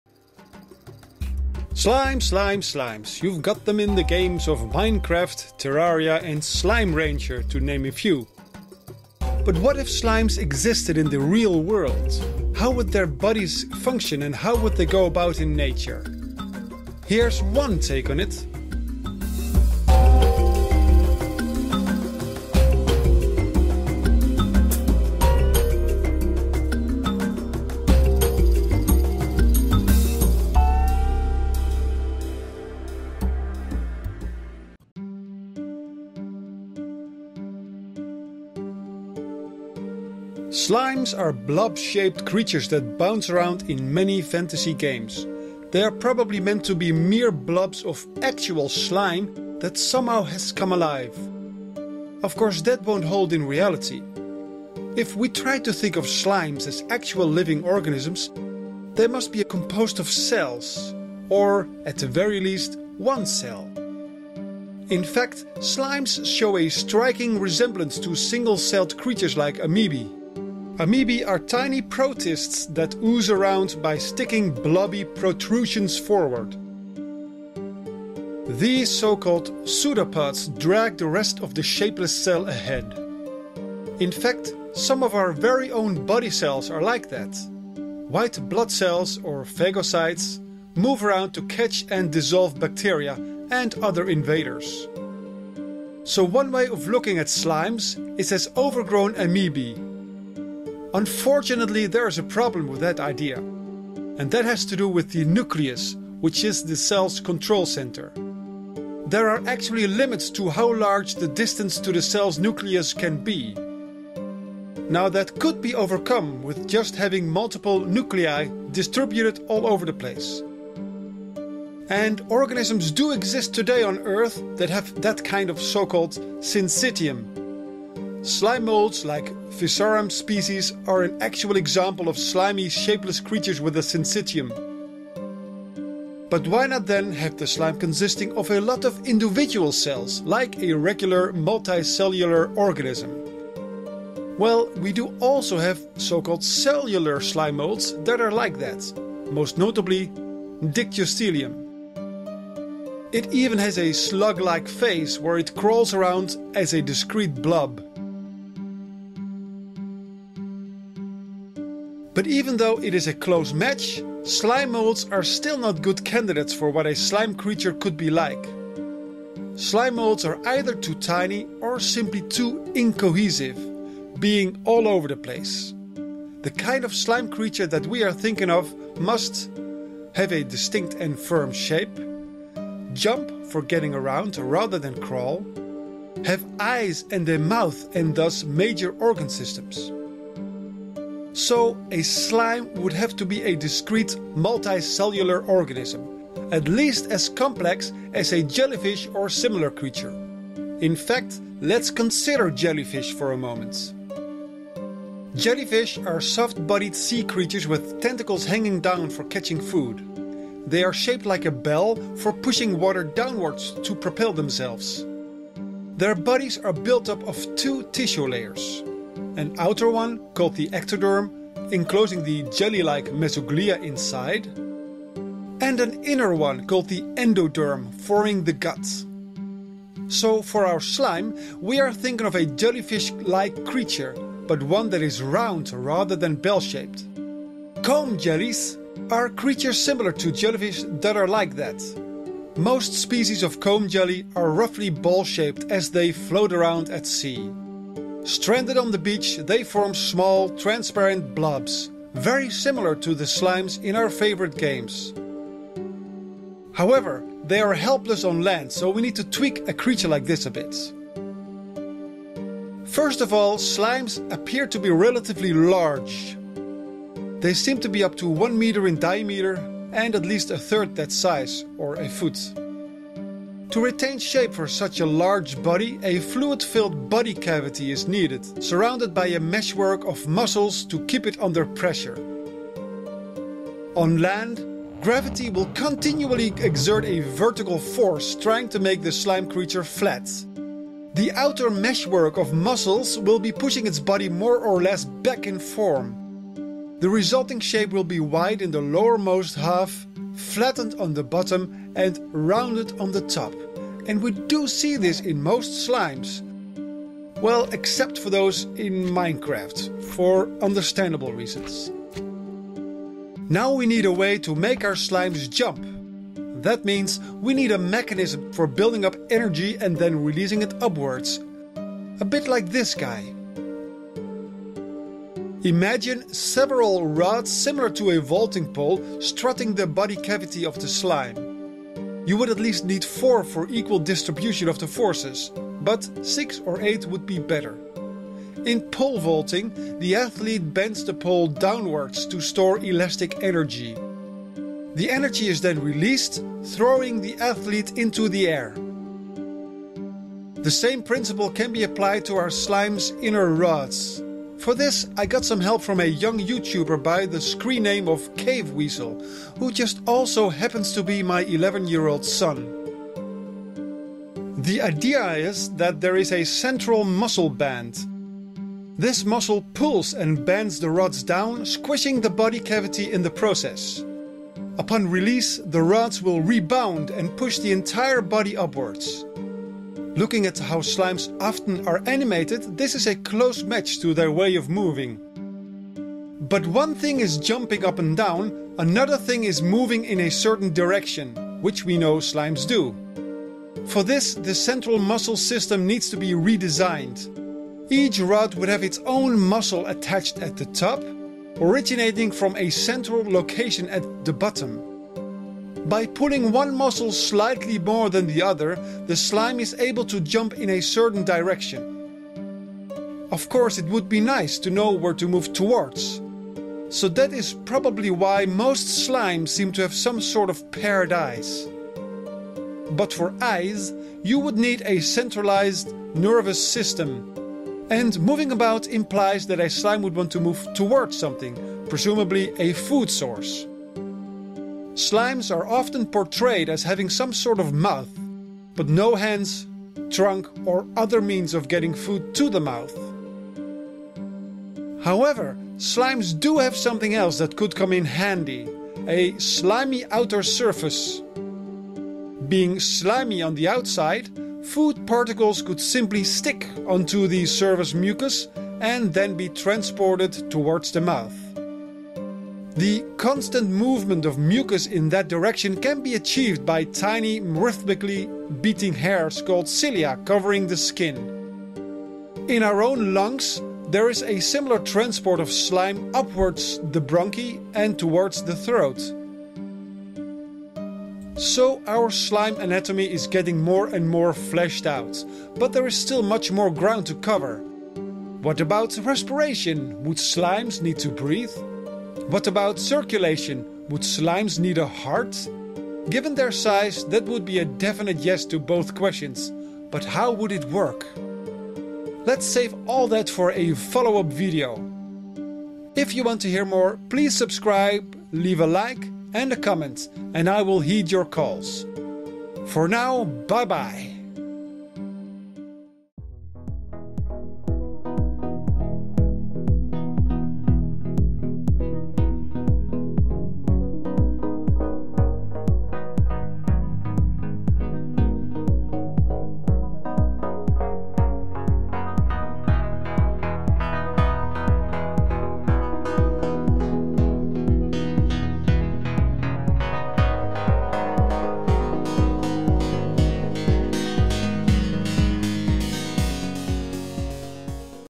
The Biology Of Slimes | Alien Analysis
Slimes are blob-shaped creatures that bounce around in fantasy games like Minecraft, Terraria and Slime Rancher. But what if slimes existed in the real world? How would their bodies function and how would they go about in nature? Here’s one take on it!

CREDITS

 - Homedrawn using paint.net